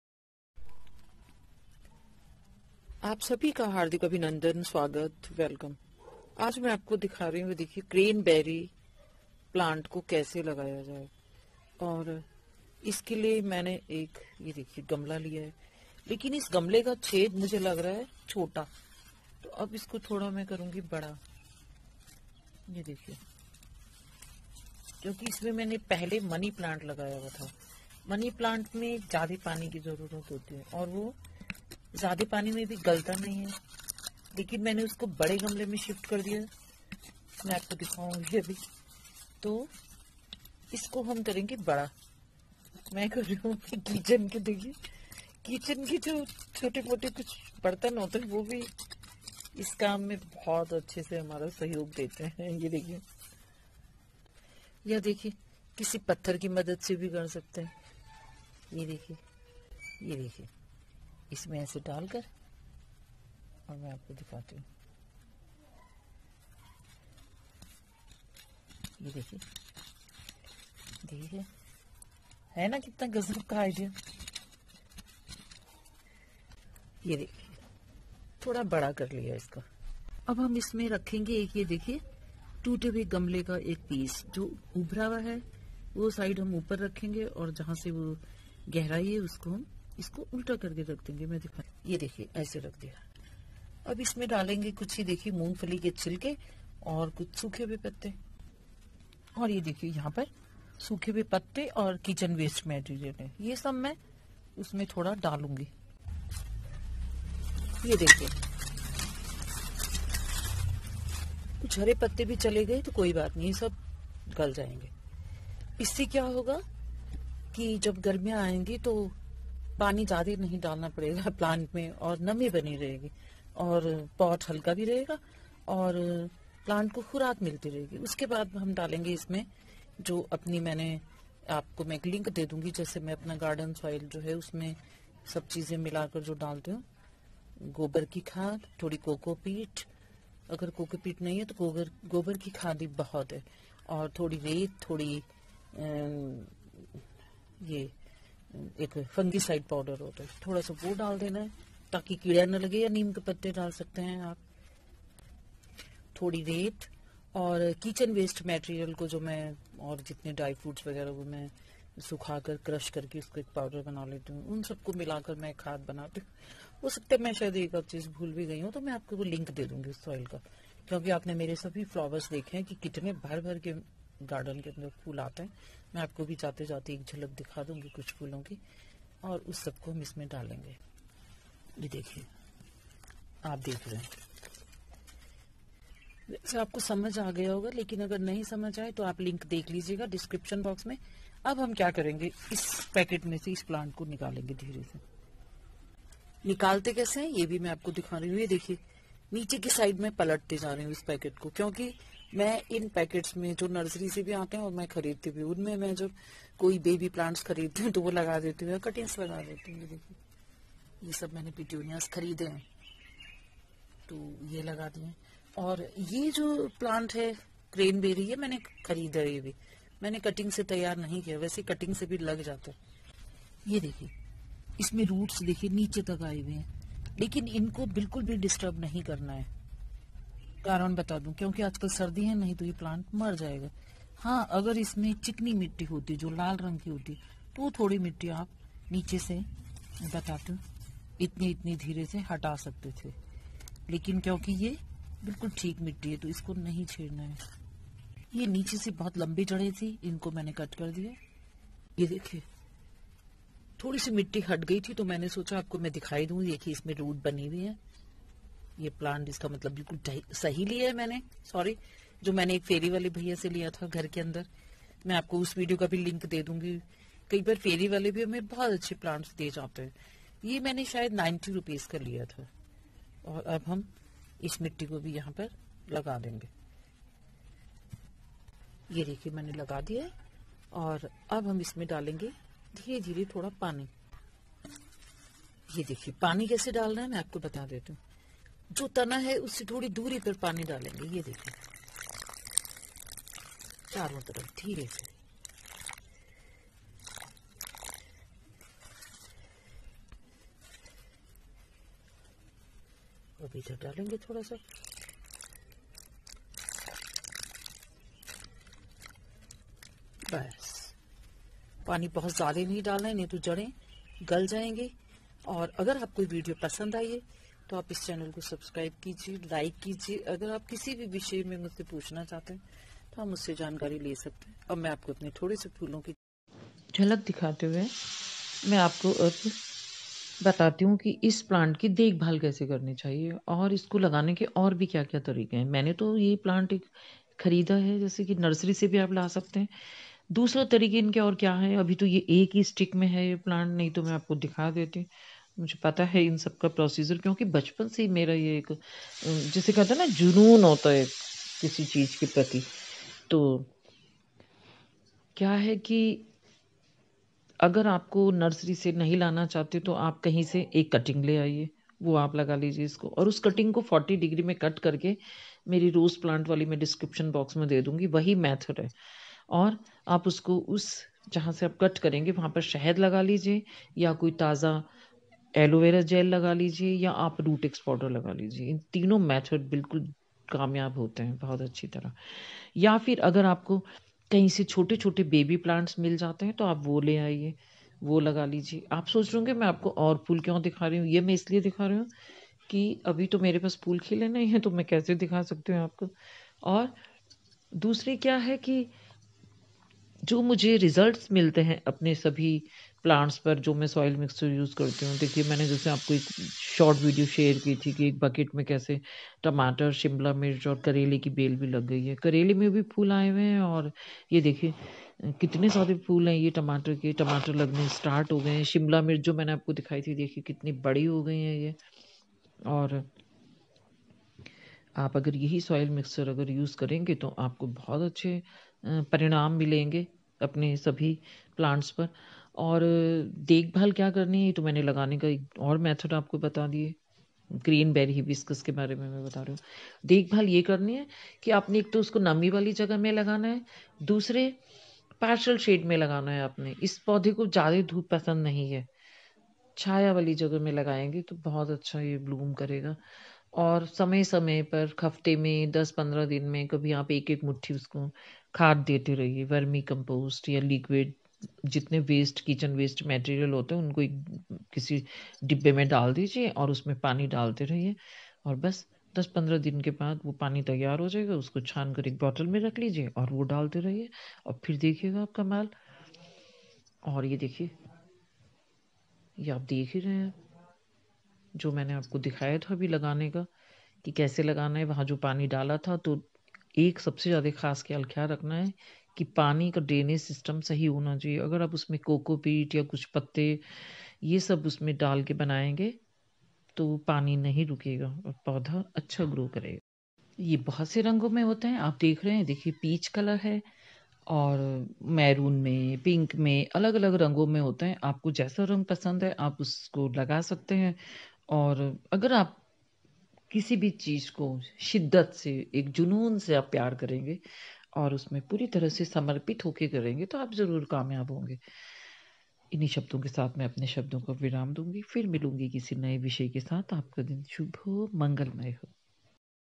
Craneberry Hibiscus ko kaise laga kar Dekhbhal karen? क्रेन बैरी हिबिस्कस का पौधा कैसे लगायें?
KaiselagayenCraneberry Plant aurDekhbhaalkaren#Howto Grow CranBerry plantandCare#Detailinformationof CraneBerryFloweringPlant